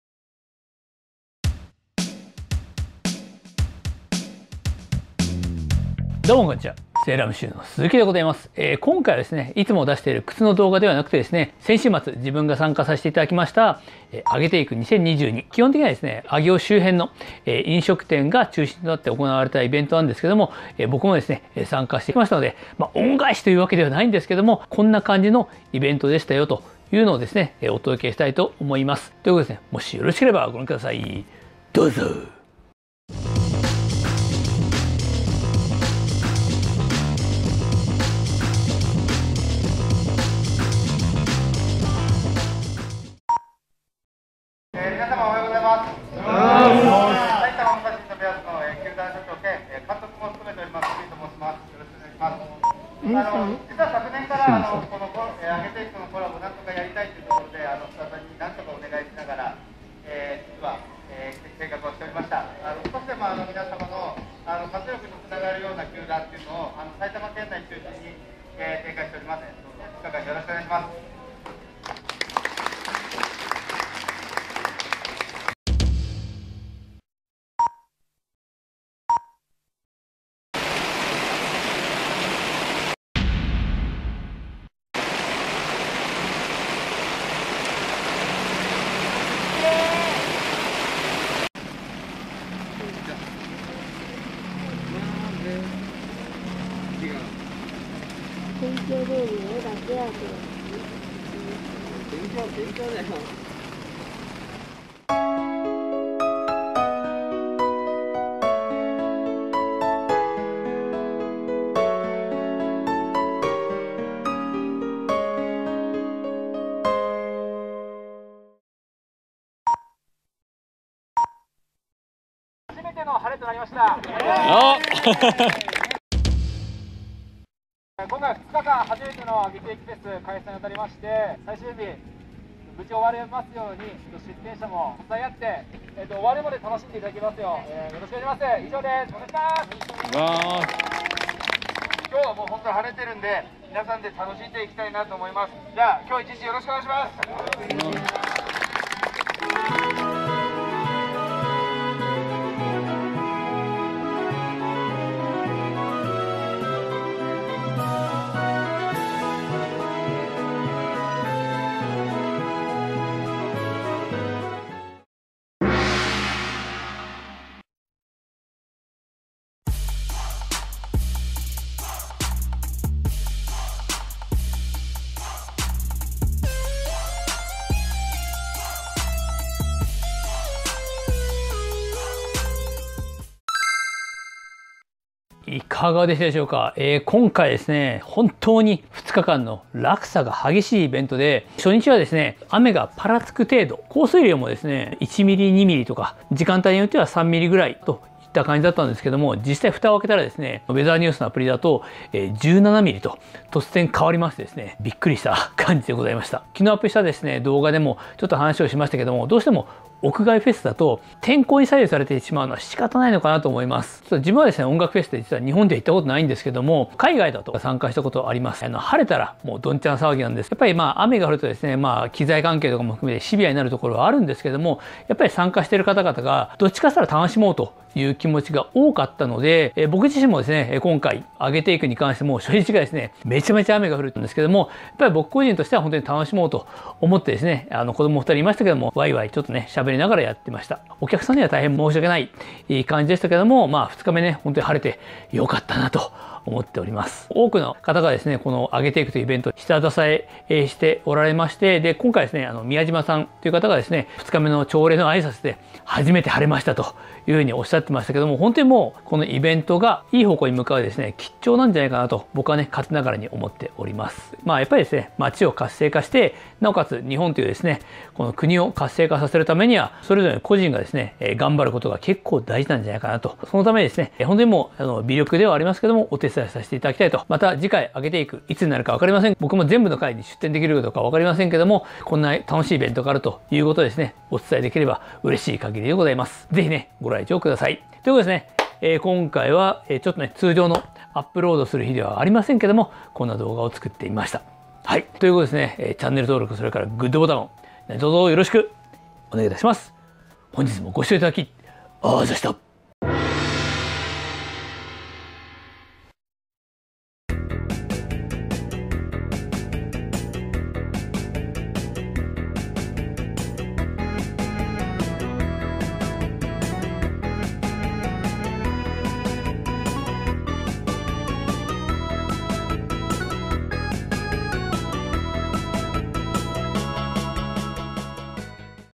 楽しい！晴れるとさらに楽しいのが屋外フェスやイベントよ
先週末出展者として参加させていただいた上尾のAGETAKEについてご紹介しますよ。
なかなかハードモードの２日間でしたが、楽しかったっす(｀･ω･´)ゞ
皆さんもお時間があればご参加くださいませ(≧▽≦)

革靴が好きで、サラリーマンを辞めて埼玉県上尾市で靴店を営んでます。
Salem Shoe Store Salem Shoe

音源：　魔王魂
イラスト：　いらすとや
地図：　マッピンドロップ